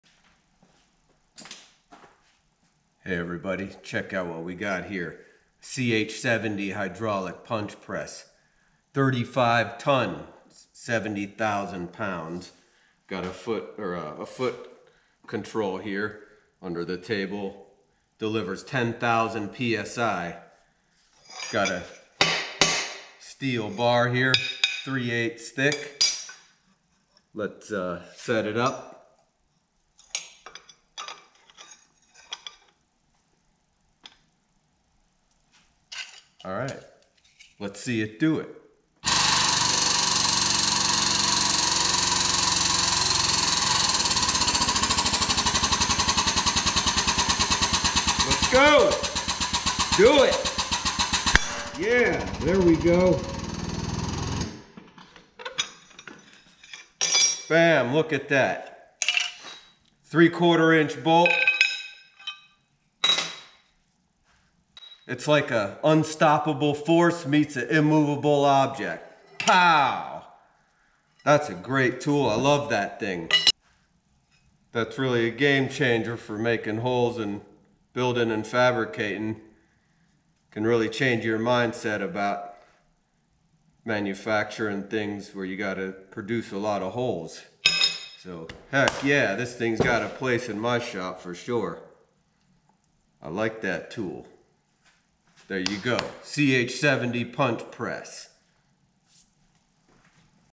Awesome Tool.CH-70 35 ton Hydraulic hole punch press review.See it in action.
In this video I show the CH-70 hydraulic hole punch punching
a(20.5 mm) 3/4 inch hole in a piece of 3/8 inch steel.